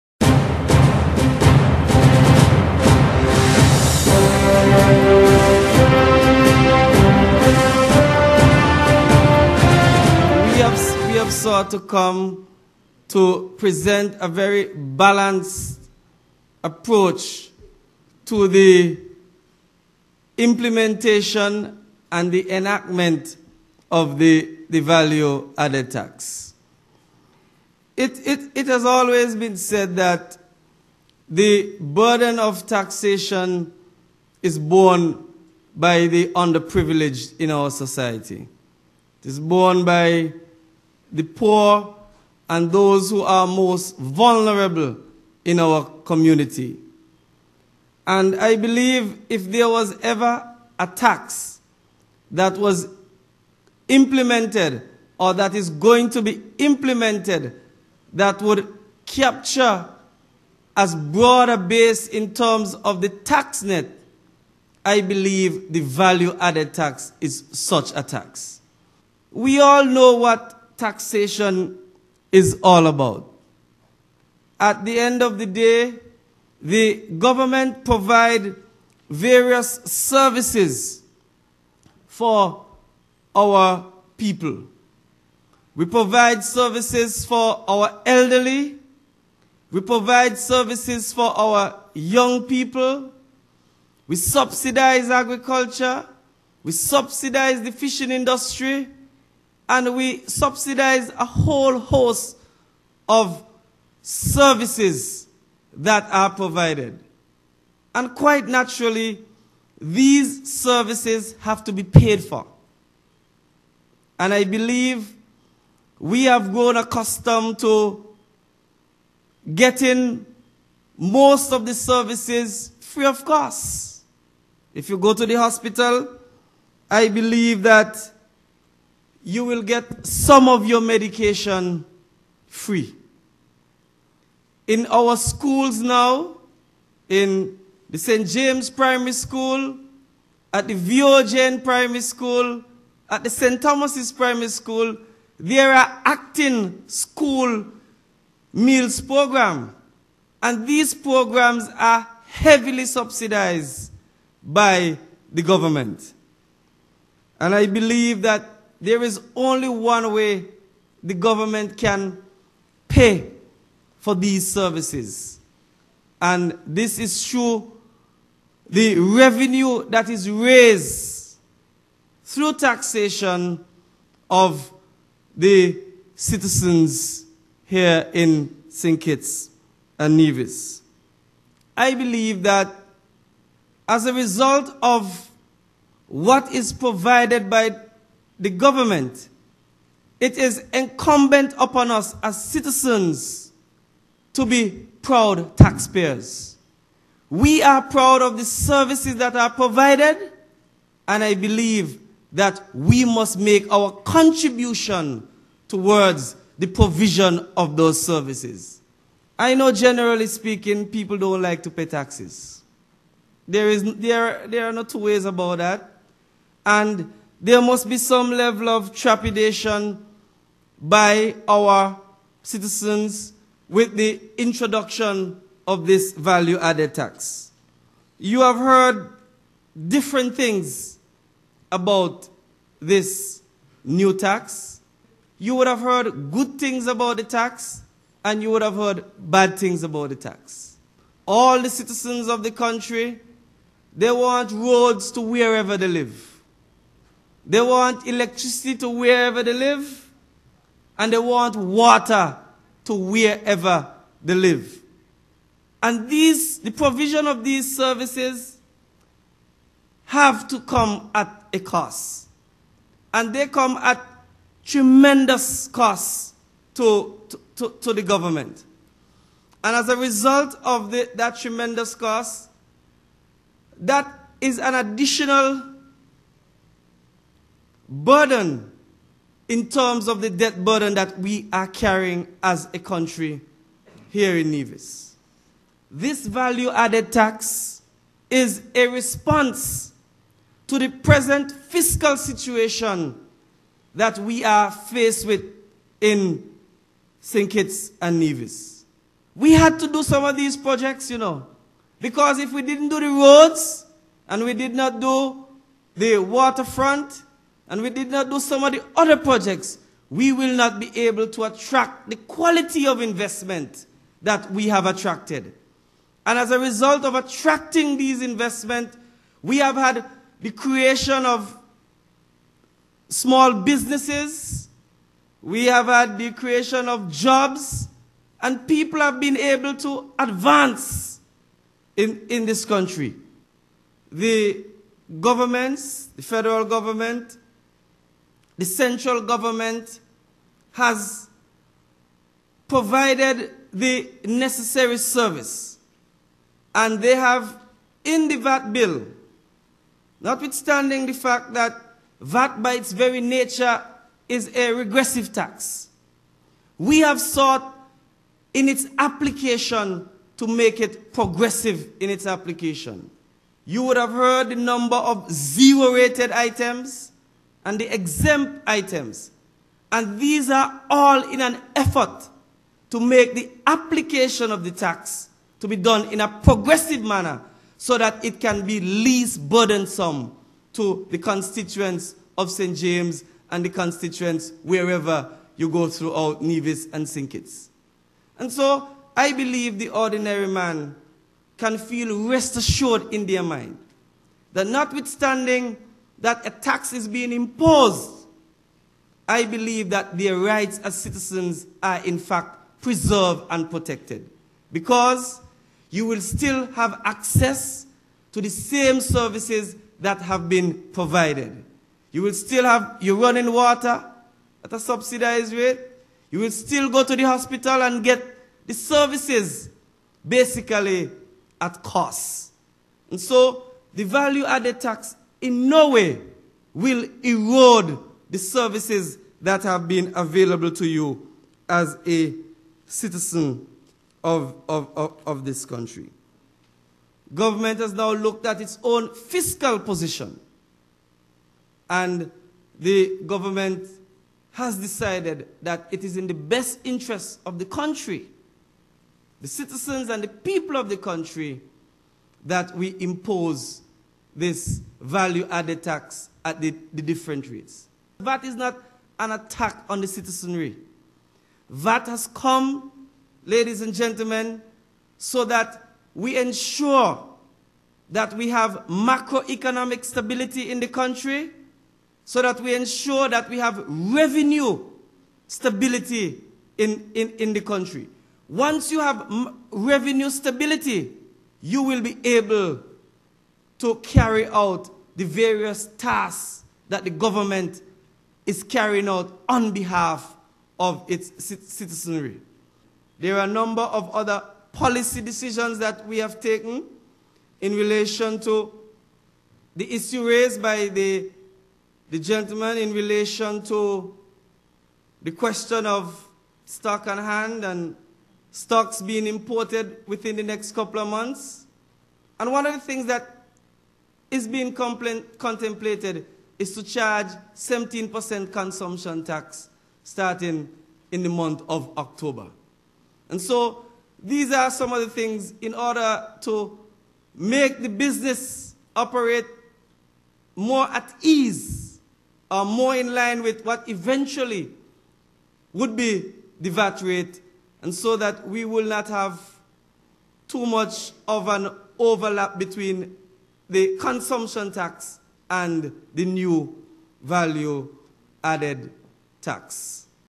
Patrice Nisbett Talks VAT in Brick Kiln
Brick Kiln, Nevis (August 24, 2010) -- Brick Kiln residents and those from neighbouring communities received an update on VAT from their Parliamentary Representative at a Town Hall Meeting on Tuesday.

The Parish MP, Attorney General, the Hon. Patrice Nisbett hosted the Town Hall Meeting, which was held at the Church of God.

The Town Hall Meeting was the first of a series that MP Nisbett will host over the coming weeks. VAT Official Kimone Moving also made a major presentation. About 60 persons attended.